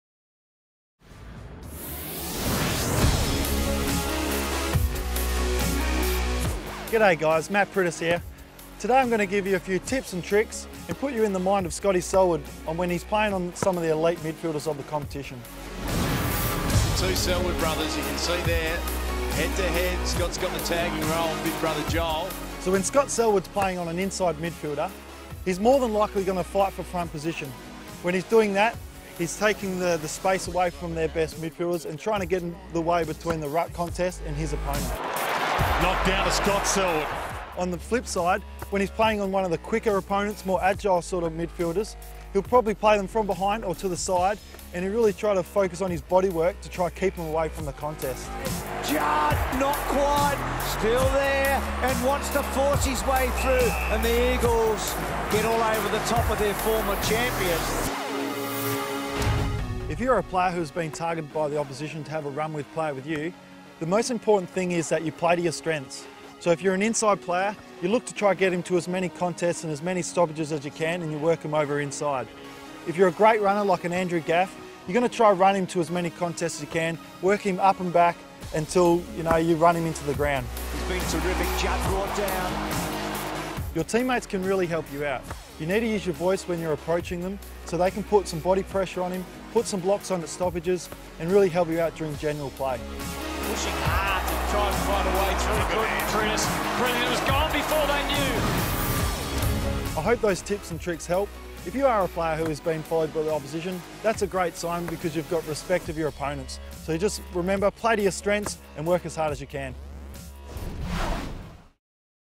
Priddis Masterclass - Tagging
The elite midfielder offers some tips on coping a tag and also running with a dangerous player for the opposition